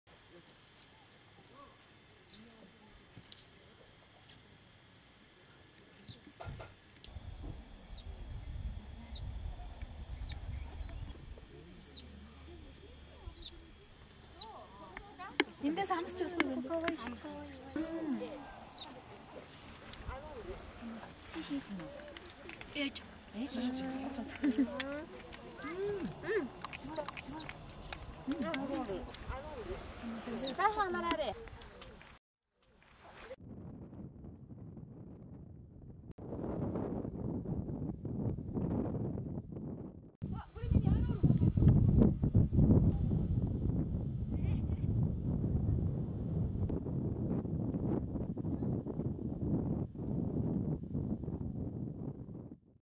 M28 モンゴルのアーロール / Mongolian dried cheese Aaruul / Ааруул
モンゴルの北、フブスグル県のフブスグル湖を訪れた時に、
モンゴルのアーロール（乾燥チーズ）を干しているところに出会いました。

アーロールは、家畜（牛、山羊、羊など）の乳を発酵させて水を切り、乾燥させて作られるチーズのような食べ物です。

家庭によって作り方や形が少しずつ違います。

撮影地：モンゴル国フブスグル県フブスグル湖
撮影日：2010年7月６日、8日

English
During a visit to Lake Khuvsgul in Khuvsgul Province, northern Mongolia. I came across a place where Mongolian aarol (dried cheese) was being dried.

Aaaruul is a cheese-like food made by fermenting the milk of livestock (cows, goats, sheep, etc.), draining it and drying it.

The method of making and the shape of them differs slightly from household to household.

Location: Khuvsgul Province, Mongolia
Date : 6 and 8 July 2010.

Монгол
Монгол улсын Хөвсгөл аймгийн Хөвсгөл нууранд очиход Ааруул хатсан газар таарлаа.

Байршил: Монгол улс, Хөвсгөл аймаг
Зураг авалтын огноо: 2010 оны 7-р сарын 6, 8.